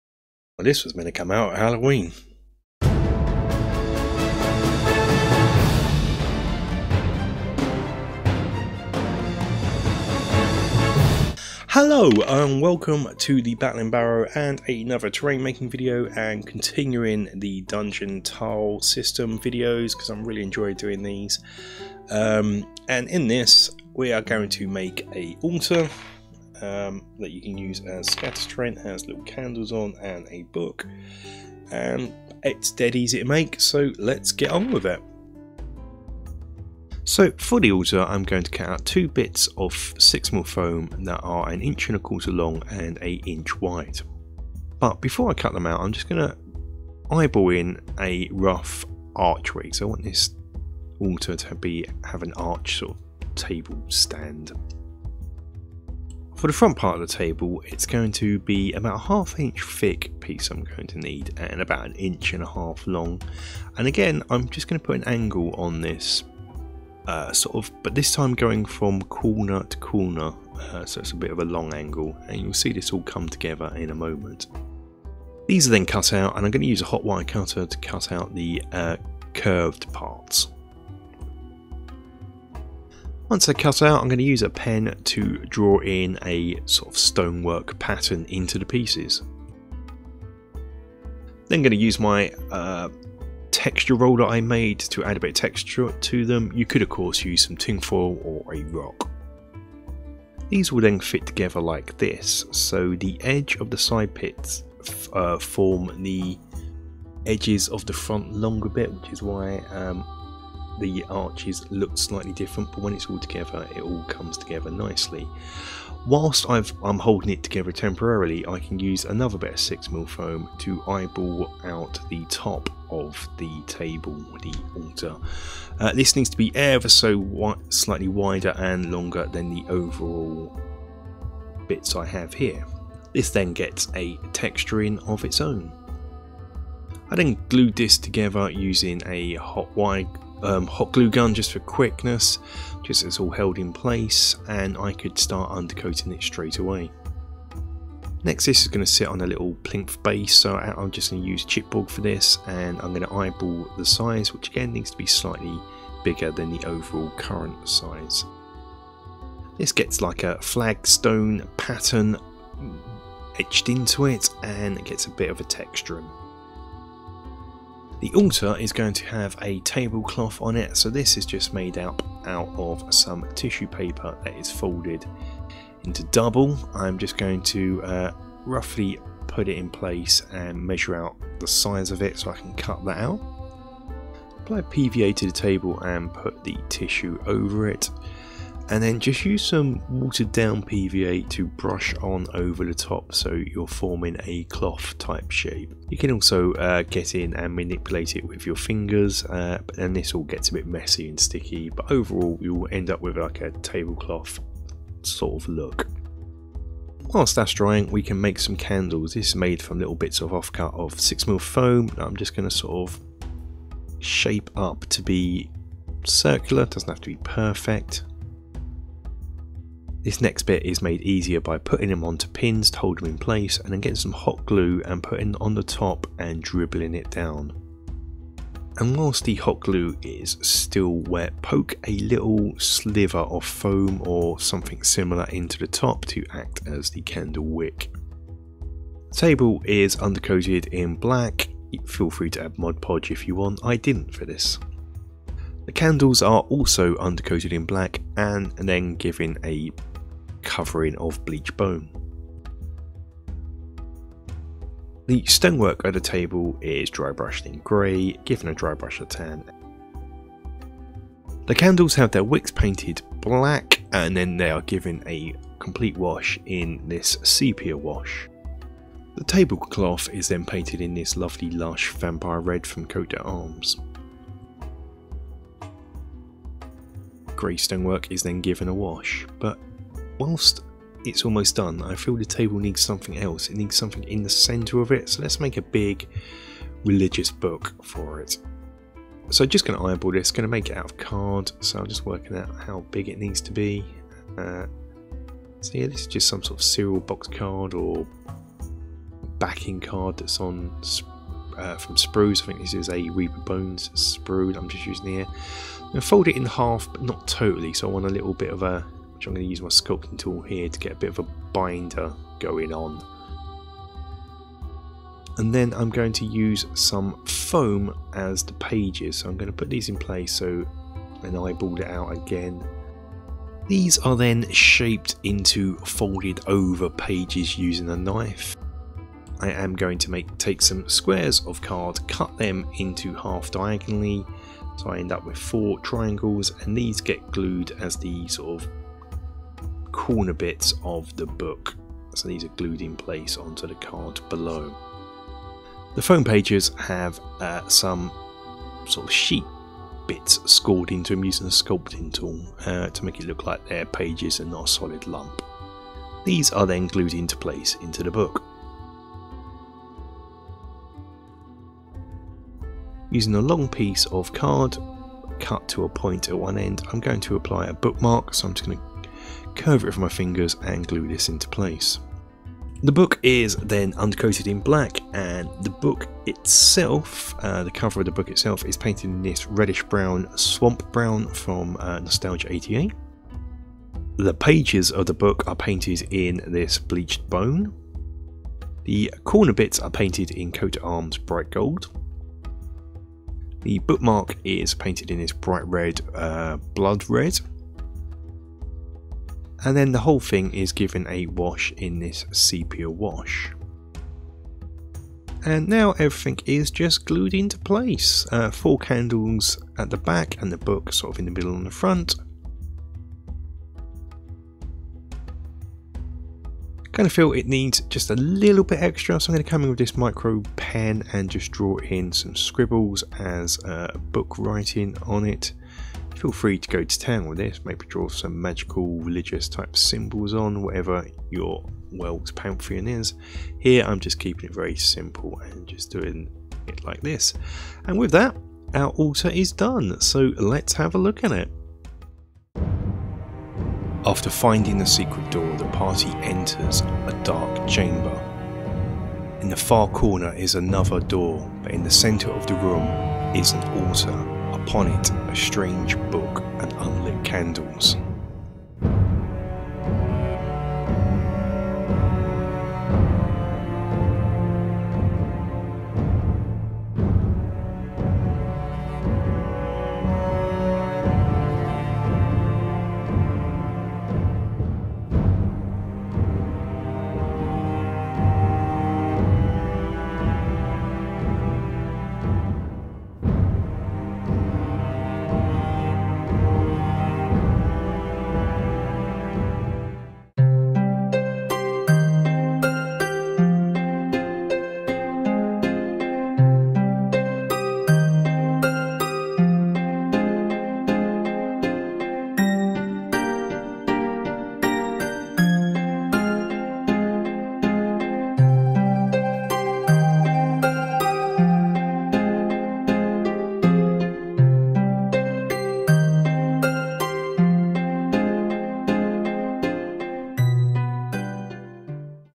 Crafting an Altar for Dungeons and Dragons Dungeon Tile Terrain
In this video I look at crafting an Altar for dungeon tiles for such tabletop rpgs as Dungeons and Dragons and Pathfinder.